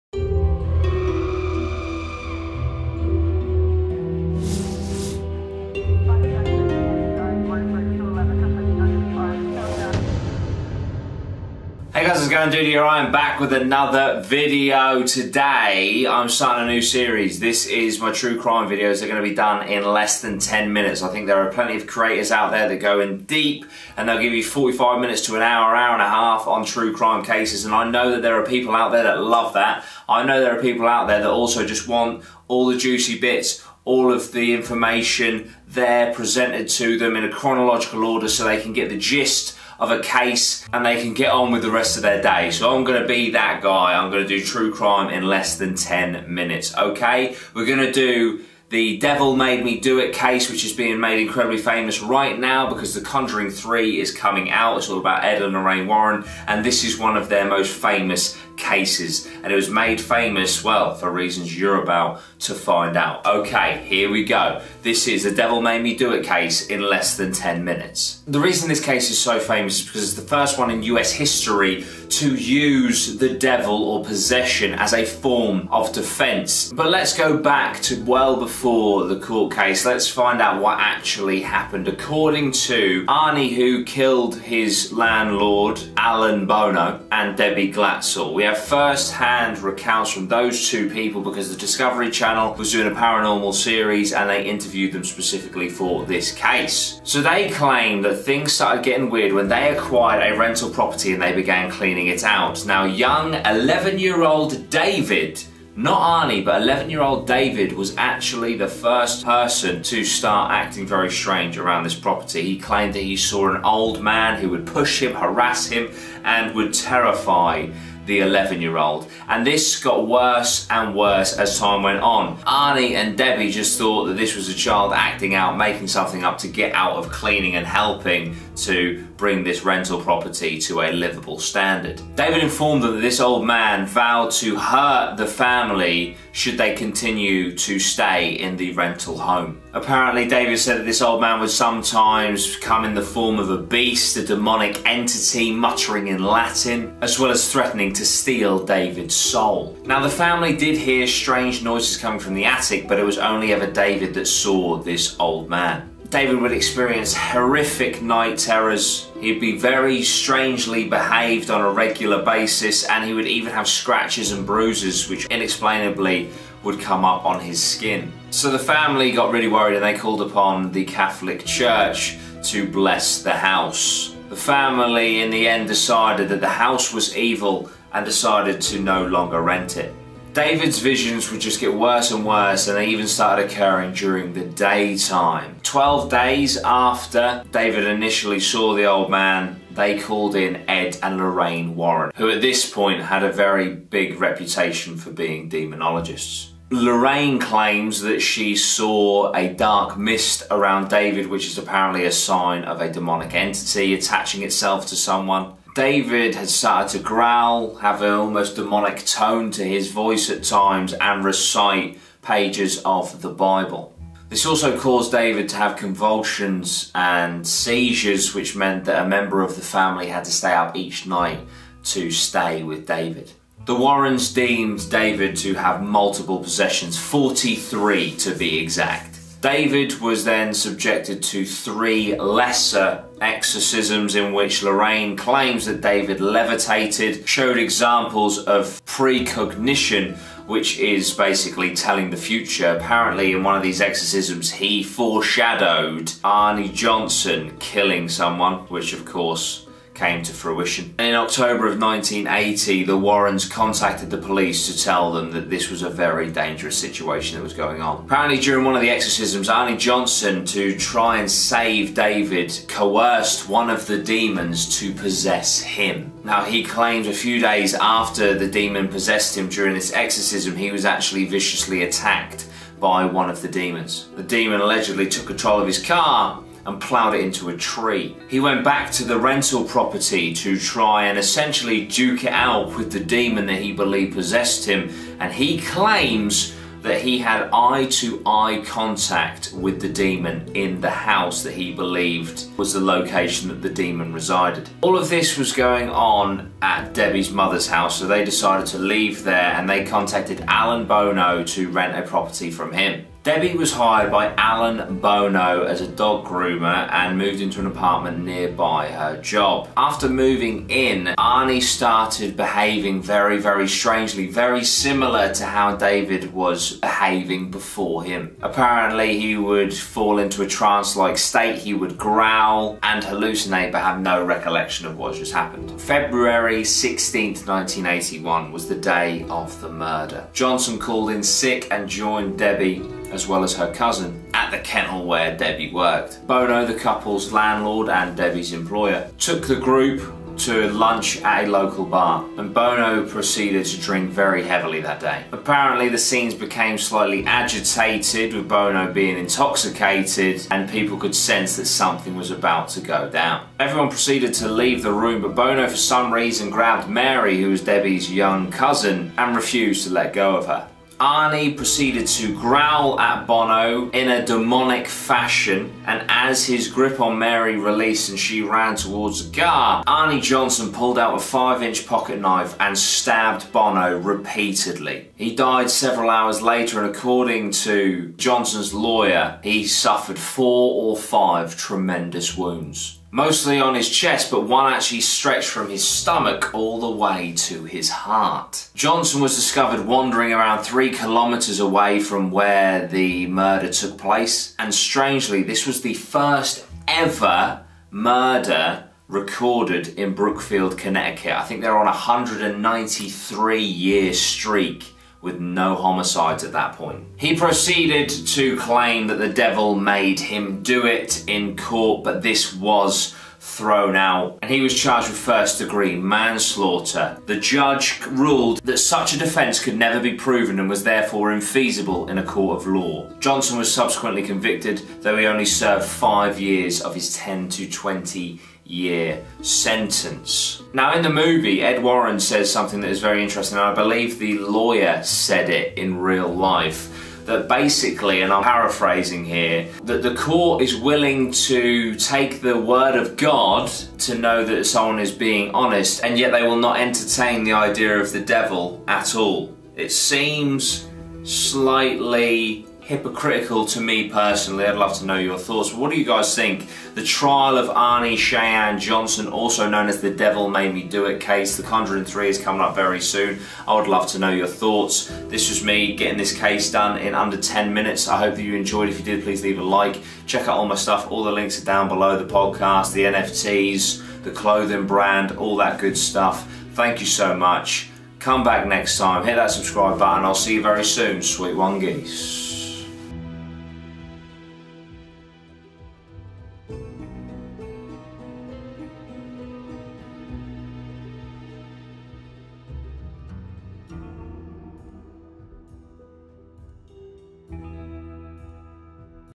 The REAL Conjuring 3 'Devil Made Me Do It' Case In Under 10 Minutes
The Devil Made Me Do It Case is the real life true crime case being used in the latest instalment in the Conjuring franchise. It is one of Ed and Lorraine Warrens most famous cases. The Devil Made Me Do It Case in under 10 minutes.

(This does not include the audio/ visual commentary by Dudey Rhino or licensed BGM).